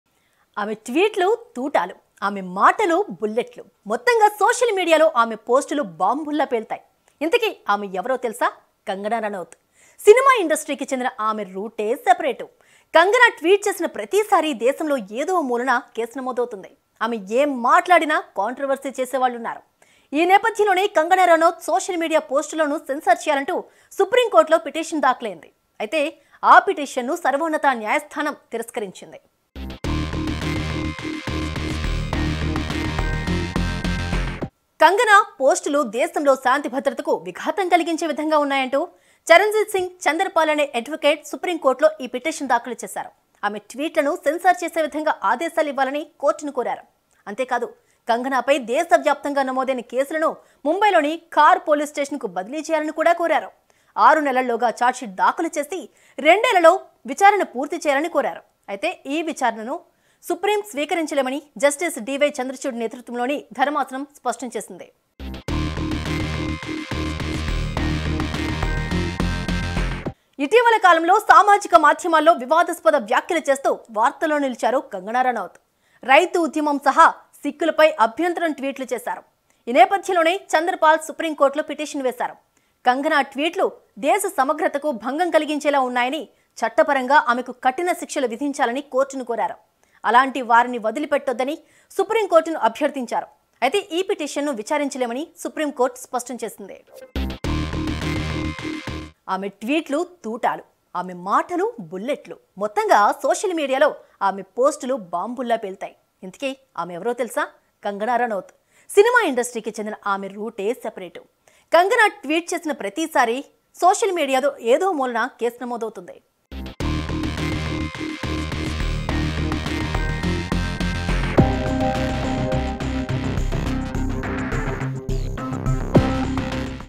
కంగనా పై కేసు ను తిరస్కరించిన సుప్రీంకోర్టు | Supreme Court on Kangana Ranaut | SumanTV Telugu
Watch► కంగనా పై కేసు ను తిరస్కరించిన సుప్రీంకోర్టు | Supreme Court on Kangana Ranaut | SumanTV Telugu